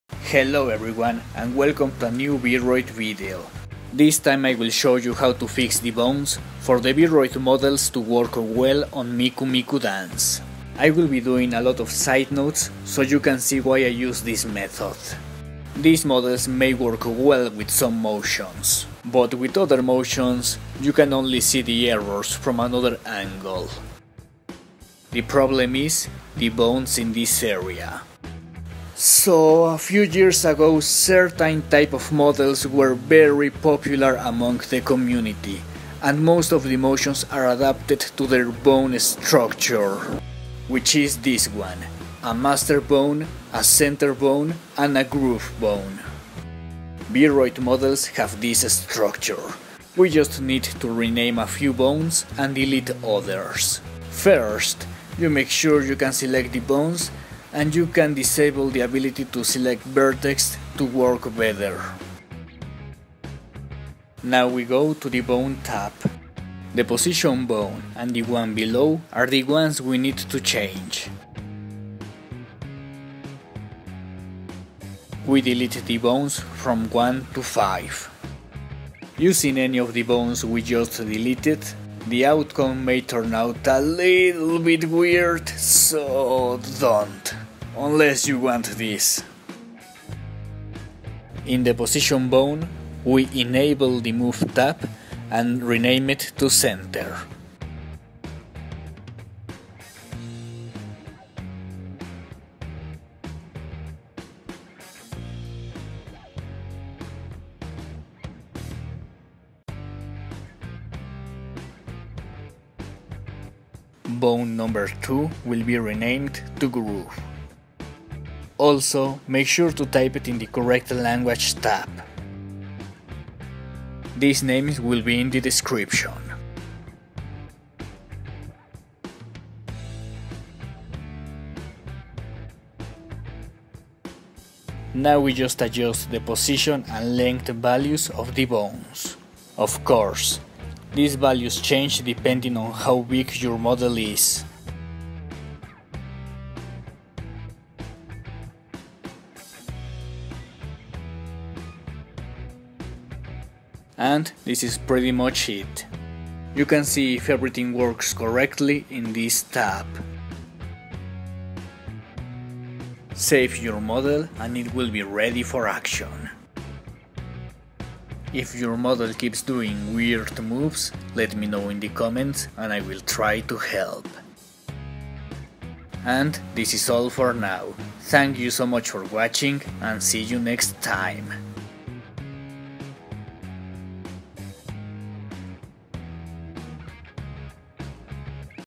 vRoid bone fix for MMD tutorial
BONE NAMES

Master: 全ての親
Center: センター
Groove: グルーブ

vRoid models may not work properly with some motions. This video shows how to fix the bones for it to work.